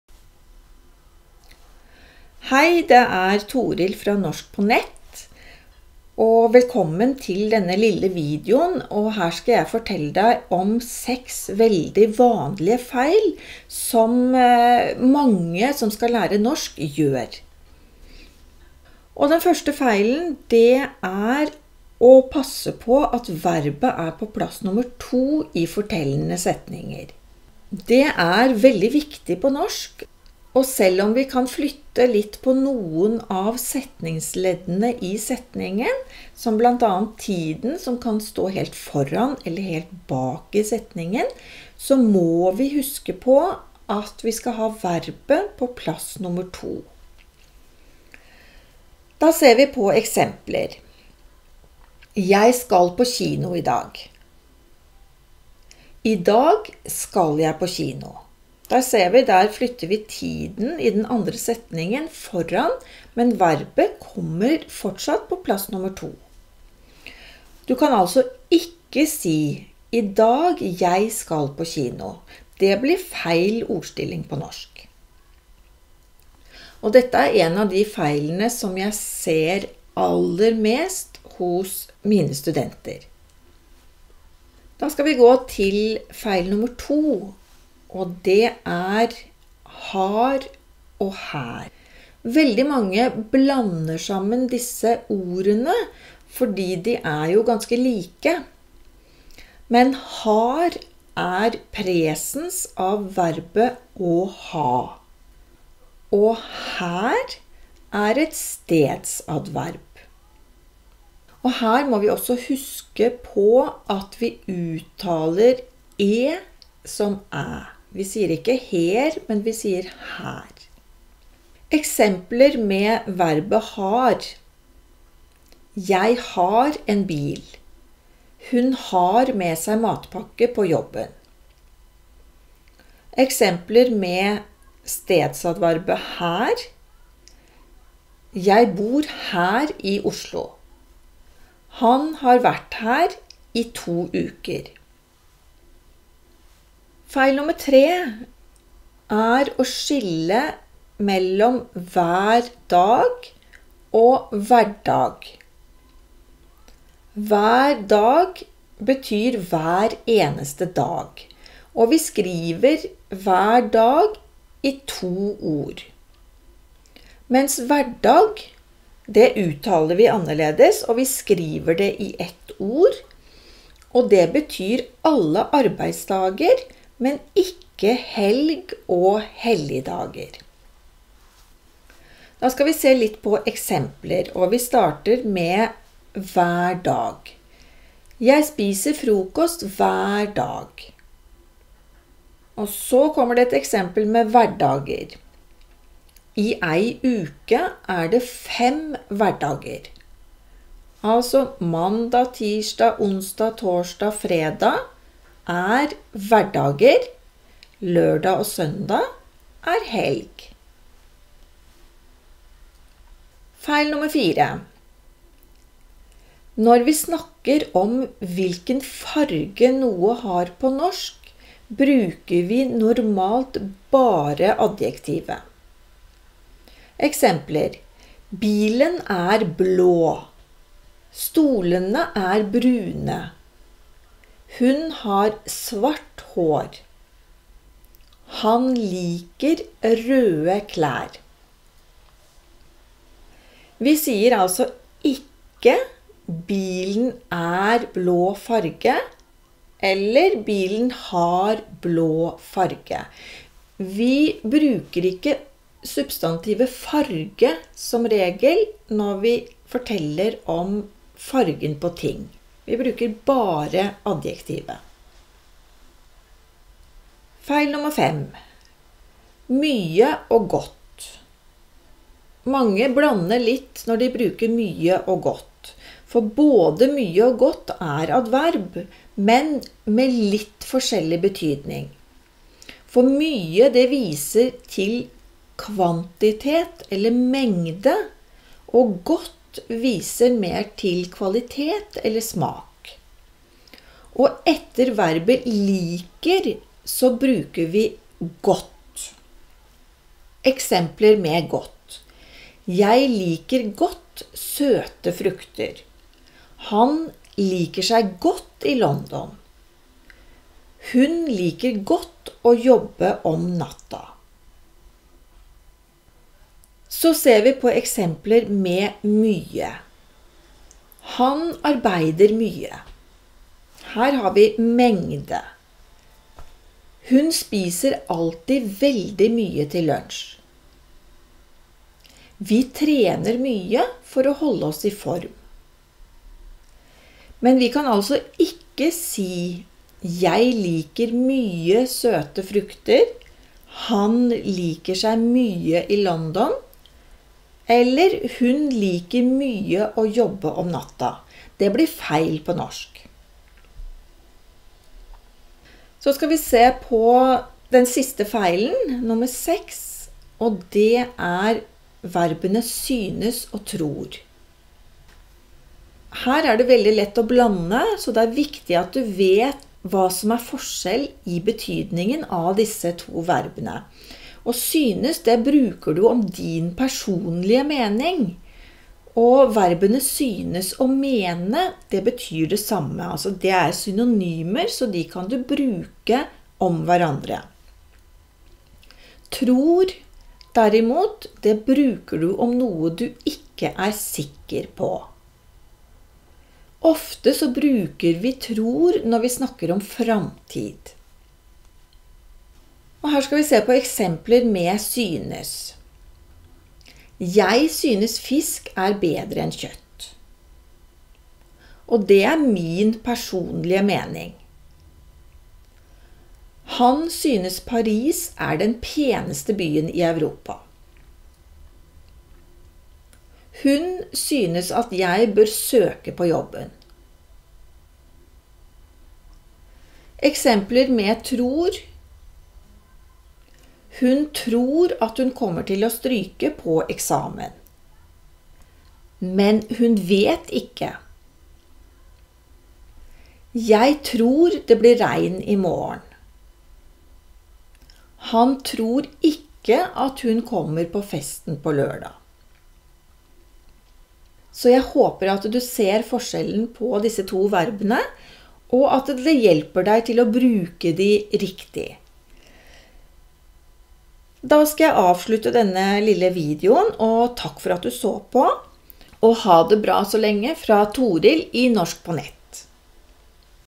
6 vanlige feil
Dette er 6 veldig vanlige feil som du bør være oppmerksom på når du lærer deg norsk.